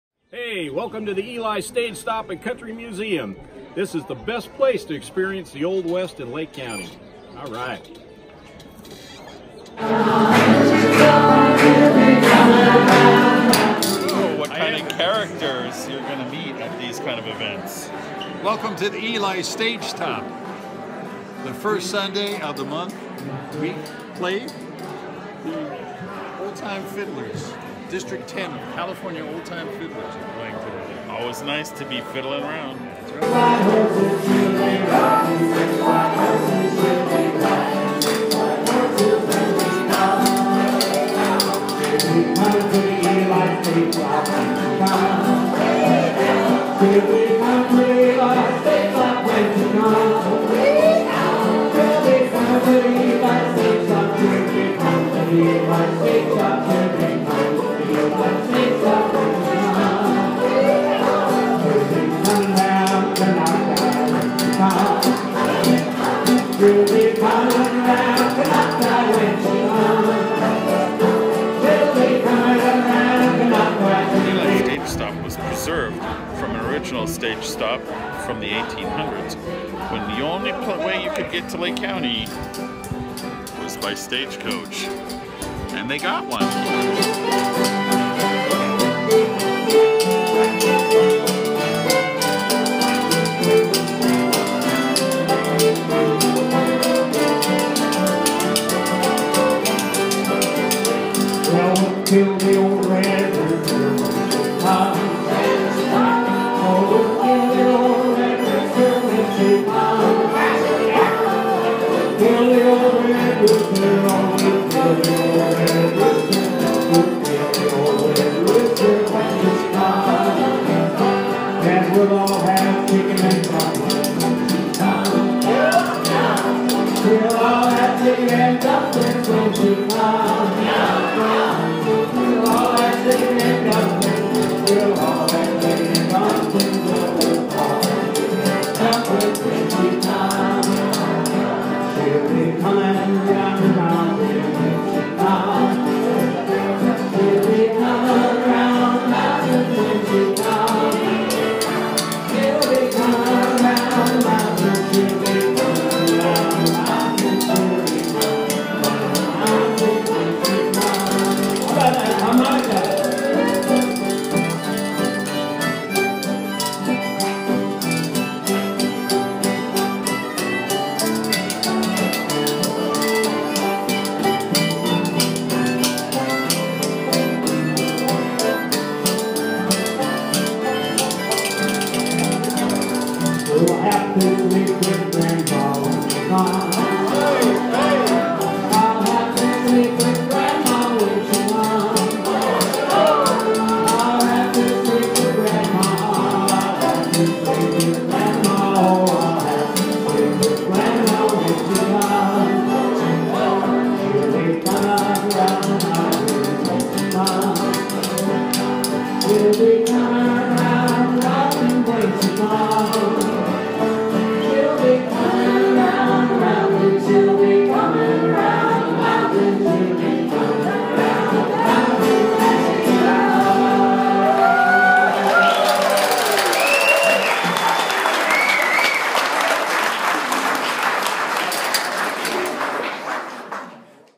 Konocti Fiddlers Jam in Lake County, CA
The Monthly Konocti Fiddlers' jam at Lake County's historic Ely Stage Stop, an original stage coach stop on the way in to Lake County in Northern California's historic wine country. The Fiddlers' jam is a fun jam session in an old barn of musicians from all over. This fun musical journey is just part of a Lake County destination vacation.